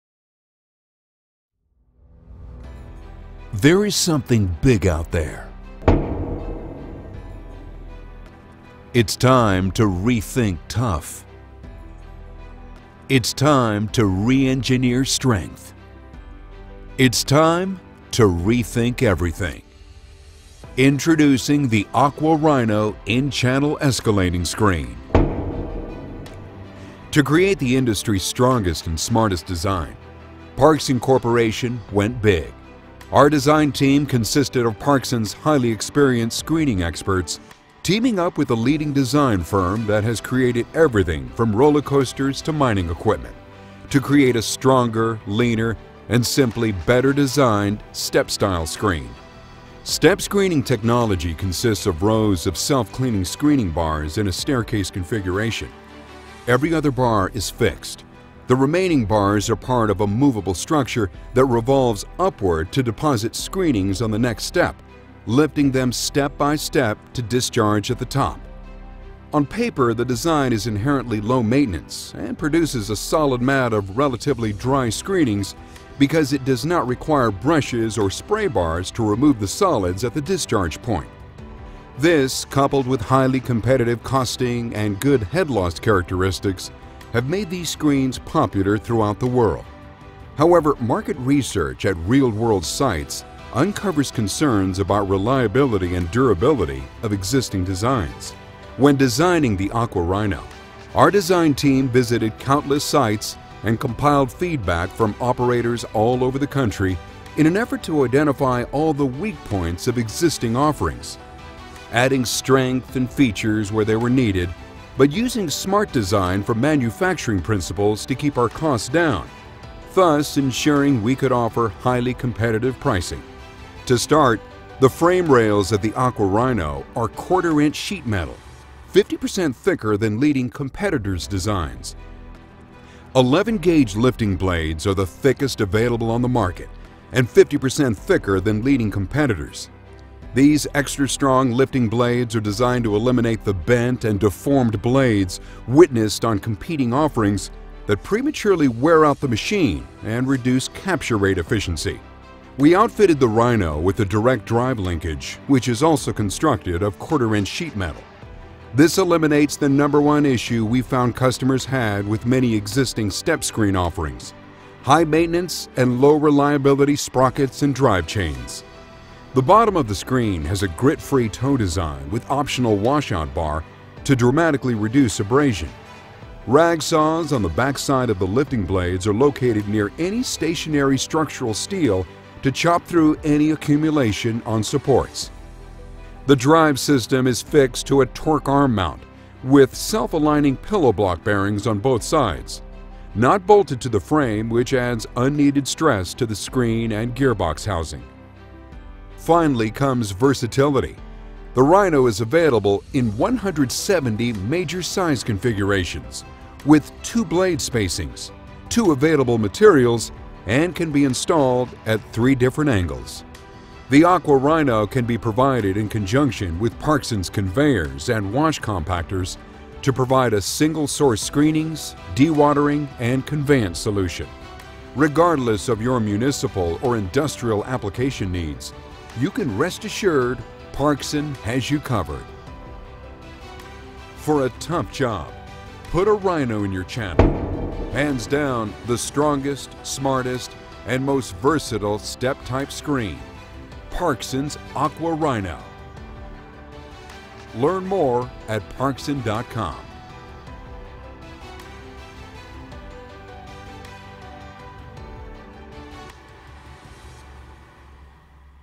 Aqua Rhino™ In-Channel Escalating Screen
The Aqua Rhino™  represents the next generation step screen, providing unmatched durability, ease of operation and ultimate protection for your downstream process equipment.

Operating Principle: Aqua Rhino Escalating Screen technology consists of rows of self-cleaning screening bars in a staircase configuration. Every other bar in this configuration is fixed, while alternate bars are part of a moveable fixture. This fixture of lift blades rotates, or revolves, upwards so as screenings are retained on the steps and form a mat, they are lifted step-by-step for eventual discharge at the top.